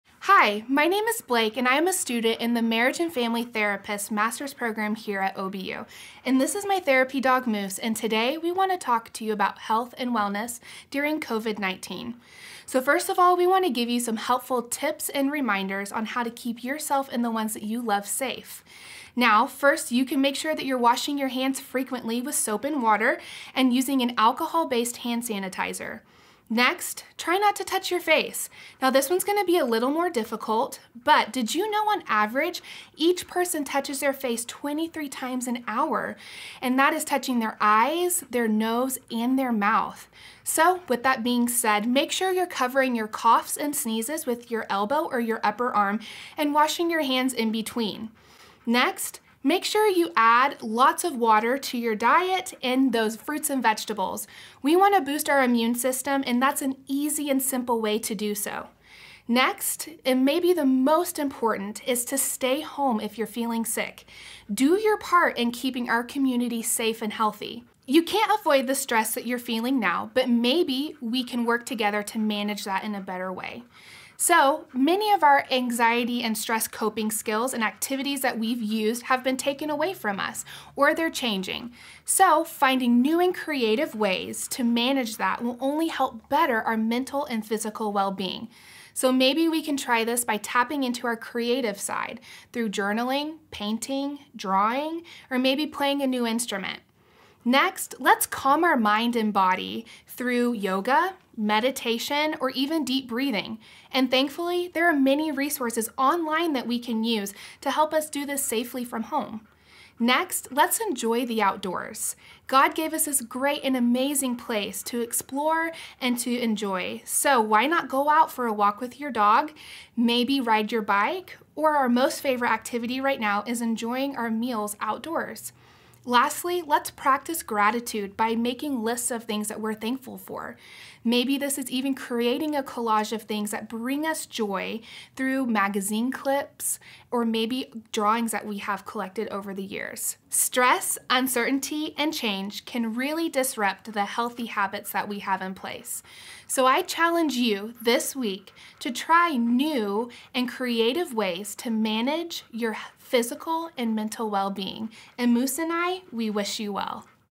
Tips on Managing Physical Health and Anxiety During Social Distancing
Managing your physical health along with anxiety is always important, but even more crucial during social distancing. Blake Kritz, a student in OBU’s Marriage and Family Therapy program, shares some reminders on how to stay well and protect others during the pandemic. She also shares tips on how to cope with stress and anxiety in some new and creative ways. This video is the fifth in a series of videos from OBU's Marriage and Family Therapy program.
Virtual telehealth appointments are available from OBU’s Kemp MFT Clinic, with students and employees receiving an annual allotment of free sessions each year.